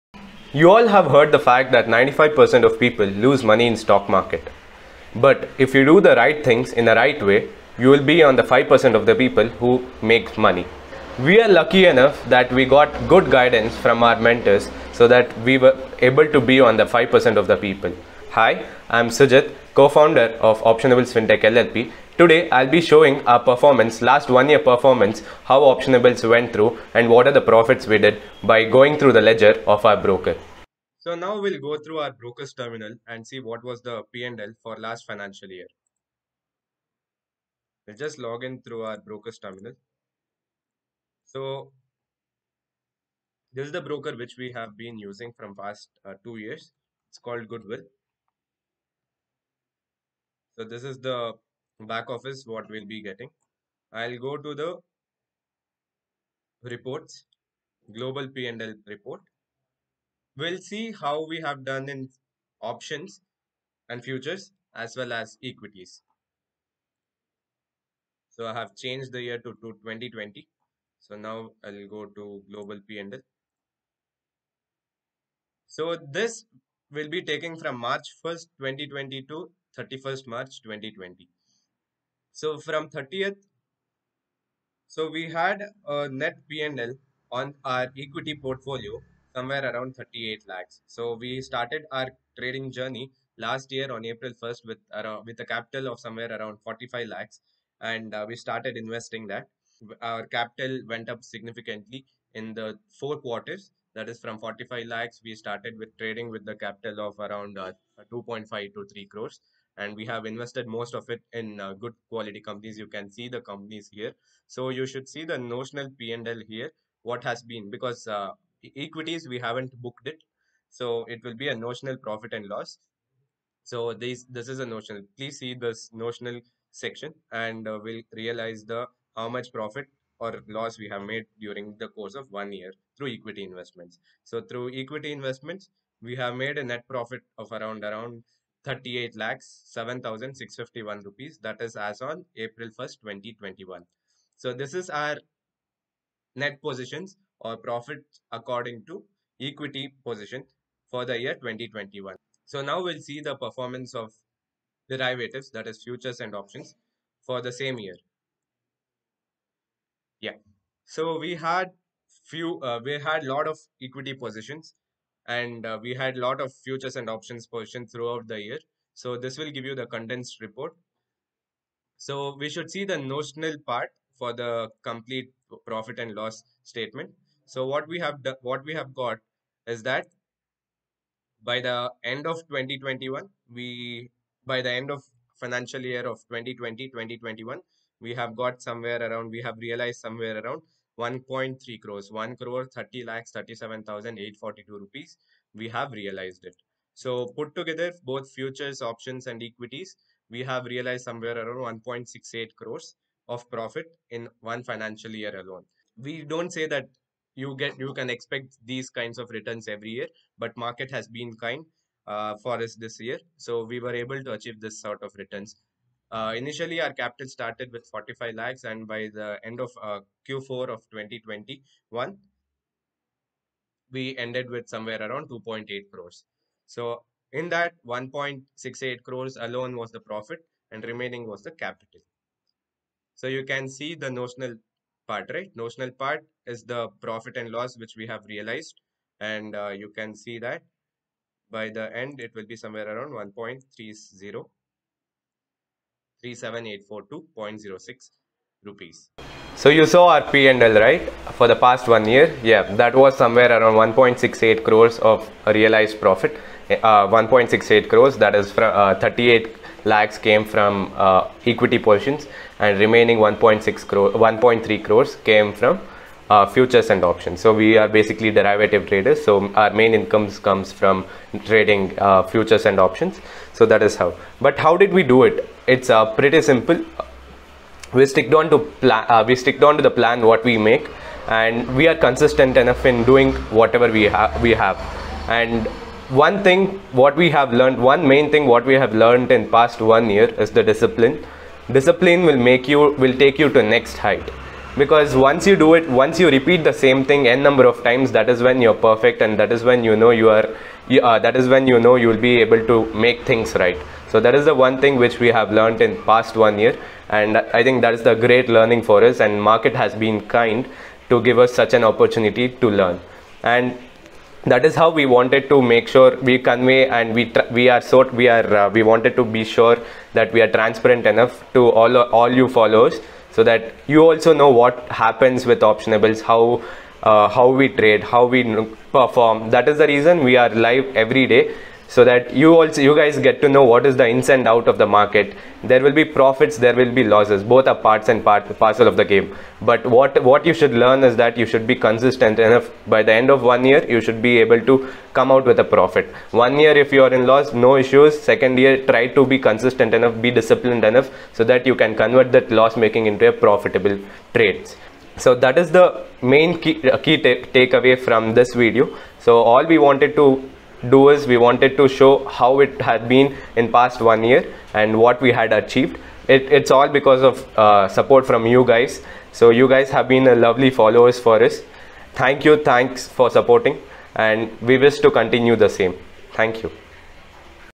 Optionables Annual Performance Review 2020-21 | 1.7 Cr Profit | 155% Return on Investment
This is a live stream of trades taken by Optionables in the proprietary trading account.

LLP's Capital as of April 1 2020: 55Lakh.

Capital Available to trade after pledging of shares around 46 Lakh

Capital as of 31st March 2021 1.8 Crs

Avg Capital through out the year 1.1 Crs